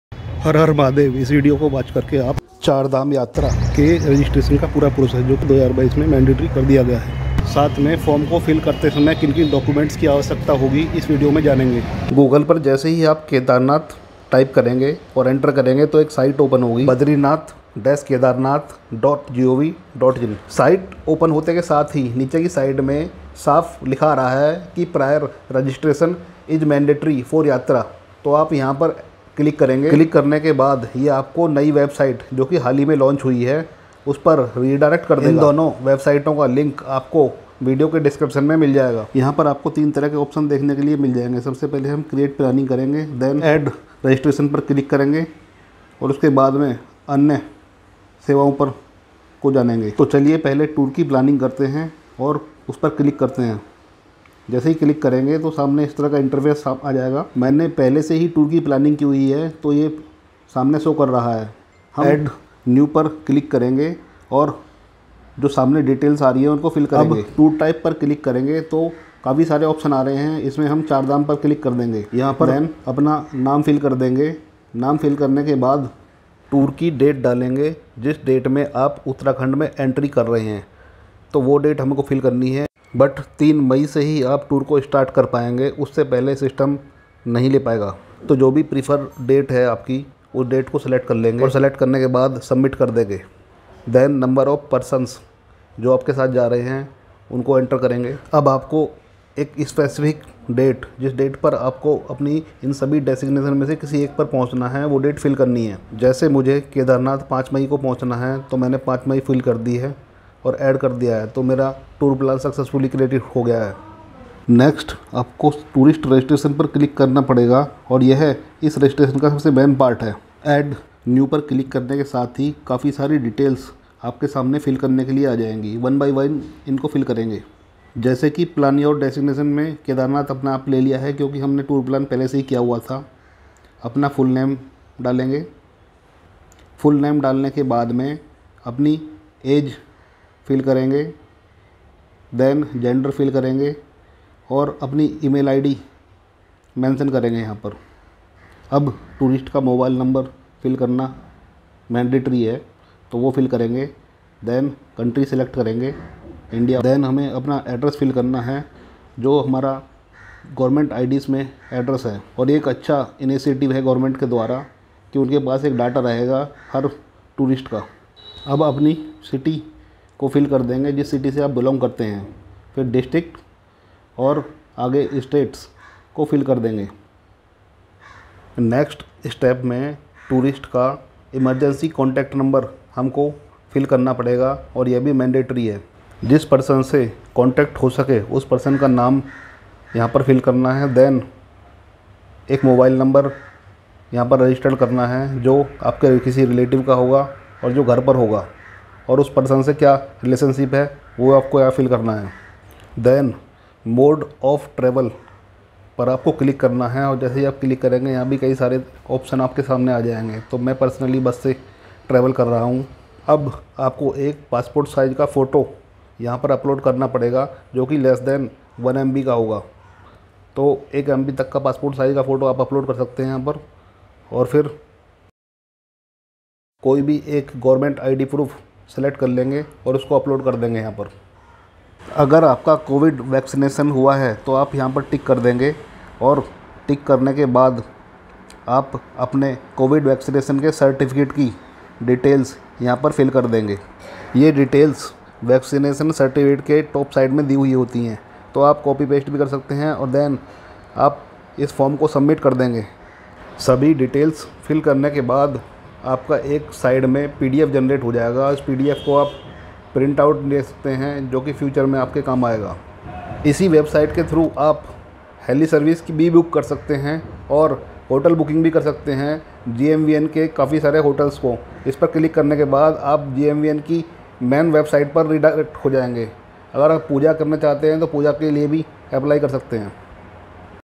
Chardham Yatra Registration 2022 | kedarnath yatra registration 2022 | kedarnath yatra kaise kare
Hello friends, today in this video I am going to tell you all about Kedarnath Yatra Char Dham Yatra new registration process also latest news & updates of kedarnath yatra in this video provided by mountainstairs.

Online Registration Website Link
https//registrationandtouristcare.uk.gov.in
Online Badrinath, Kedarnath Jii Arti Booking
htttps:// badrinath-kedarnath. uk.gov.in